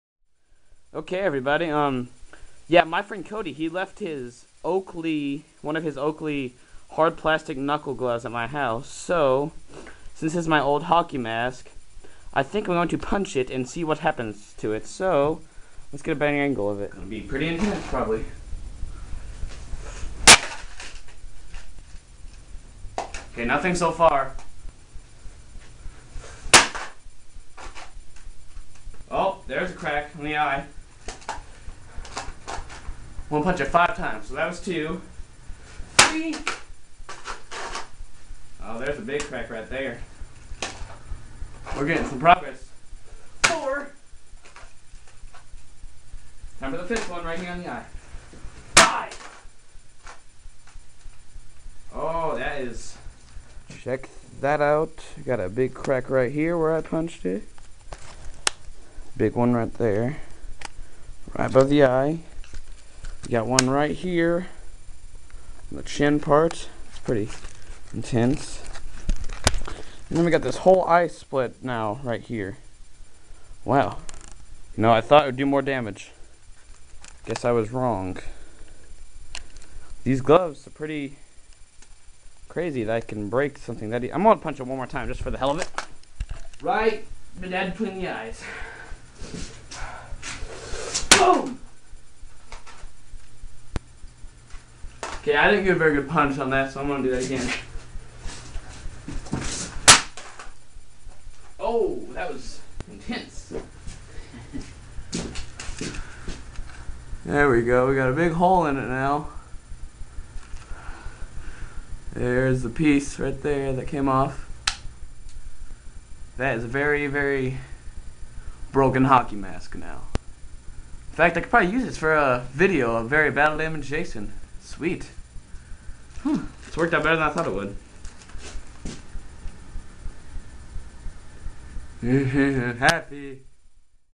Destroying an old Jason Voorhees Hockey Mask
The title explains it...and so does the video...